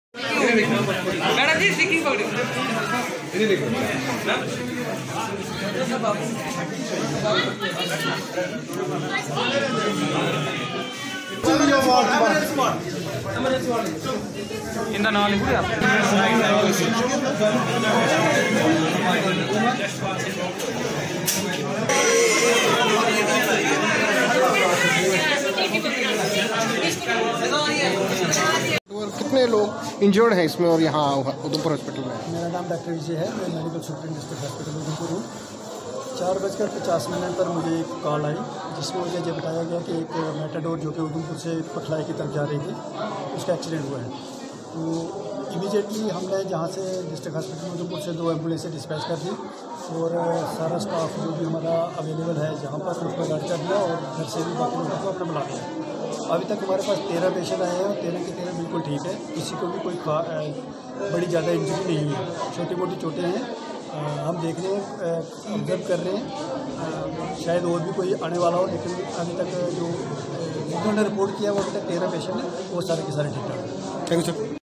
A Mini Bus accident on Barmeen road. 13 passangers got injured and admiitted in district hospital u
A Mini  Bus accident on Barmeen road. 13 passangers got injured and admiitted in district hospital udhampur for furthet treatment.